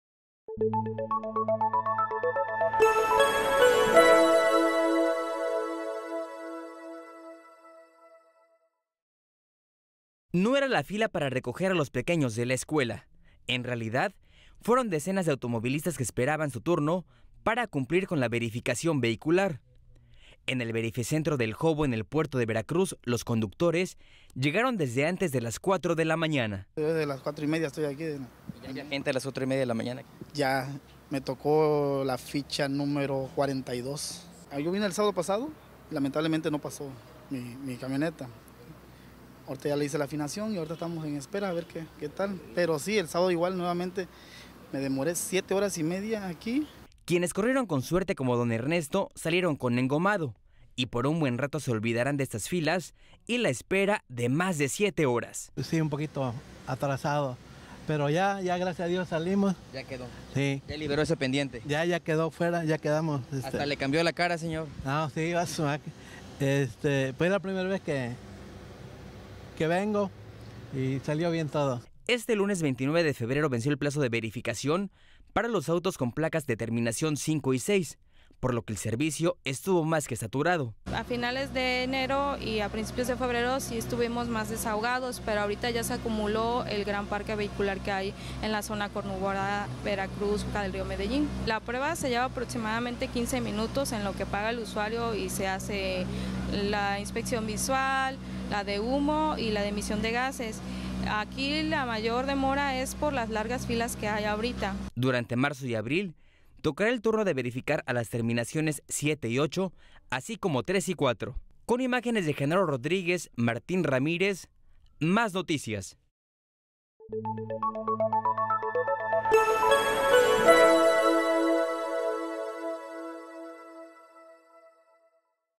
No habrá multas durante primer semestre de la verificación vehicular
La Secretaría de Medio Ambiente (SEDEMA) invita a los veracruzanos a verificar sus vehículos, e informa que durante el primer semestre del Programa de Verificación Vehicular no habrá multas.